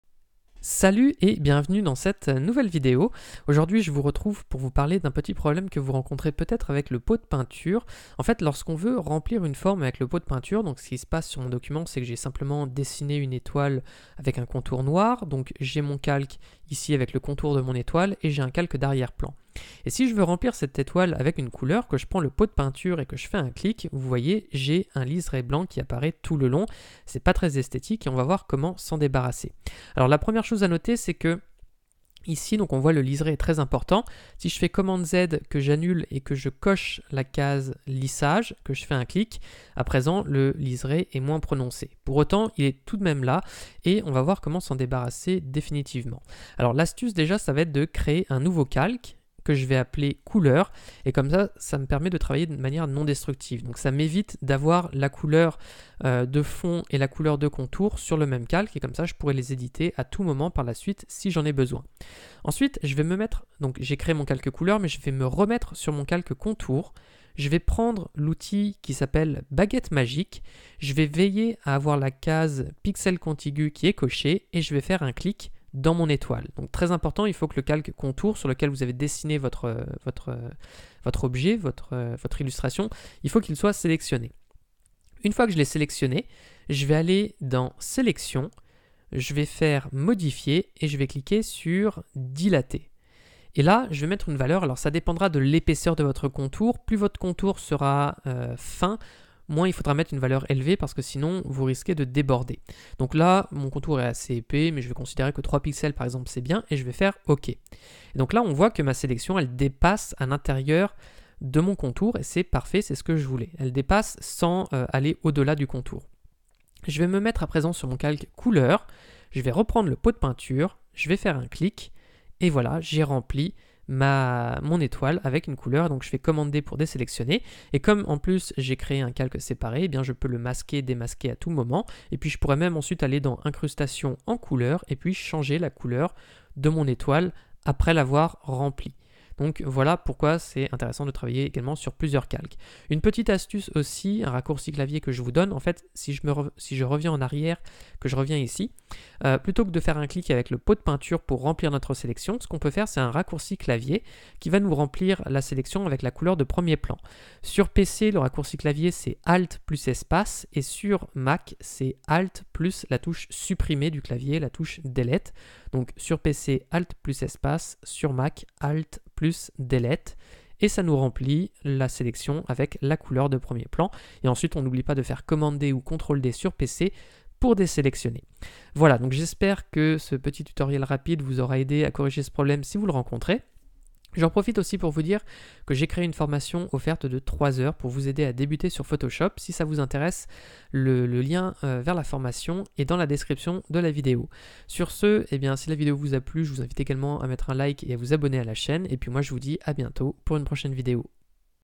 Comment bien utiliser le Pot de Peinture - Tuto Photoshop
👉 Je te montre comment remplir correctement une forme avec le pot de peinture, et éviter de te retrouver avec une bordure blanche pas très esthétique.

👉 La meilleure banque d’images pour des ressources graphiques illimitées (Envato Elements)

▸ Comme d'habitude, si tu as des questions, j'y réponds en commentaire !

▸ Si tu apprécies cette vidéo, n'hésite pas à la partager !